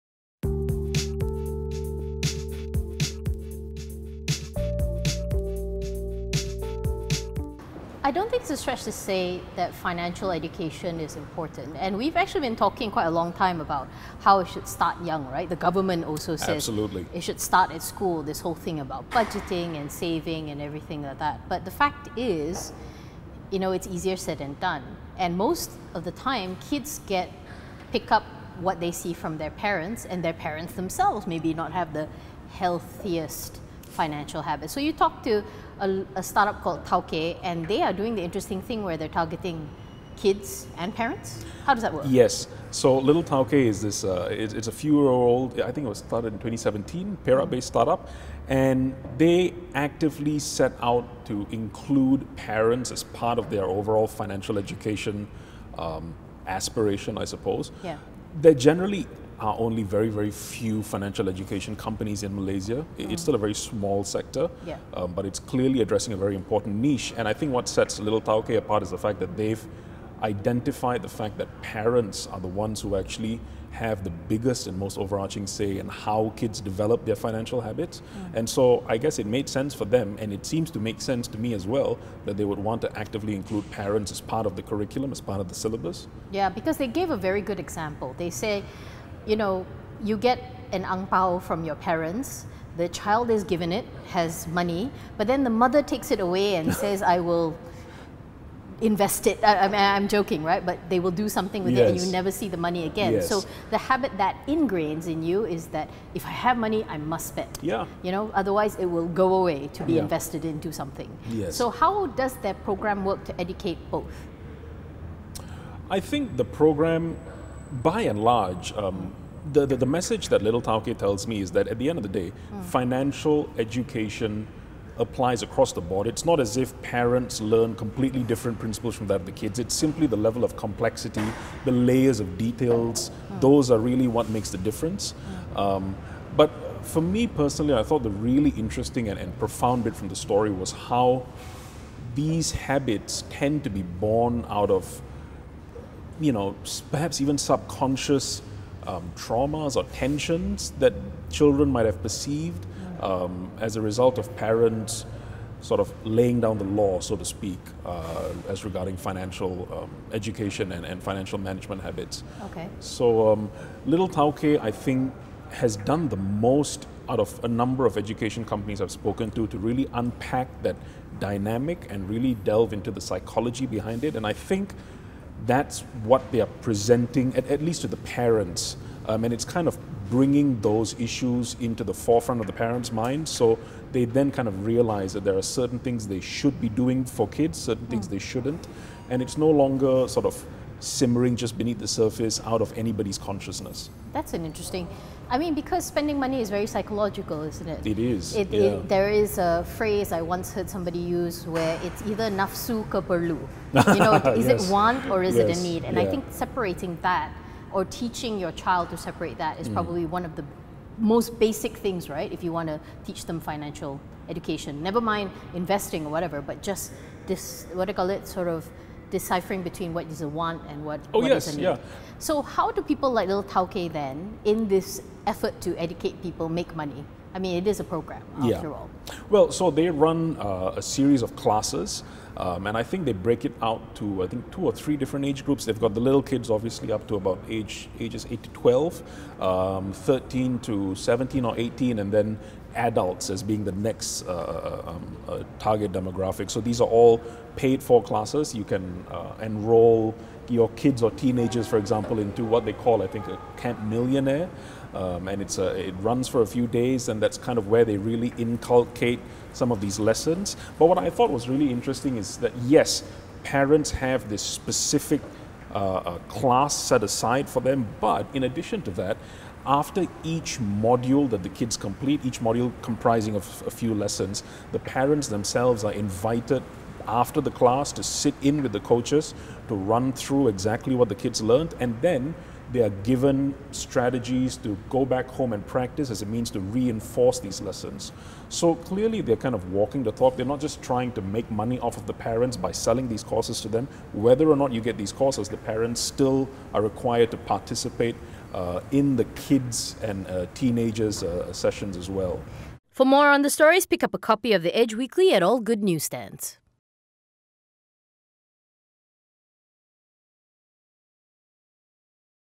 BEHIND THE STORY: The importance of good financial parenting
While most agree that financial education should start from young, should parents also be brought back into the classroom?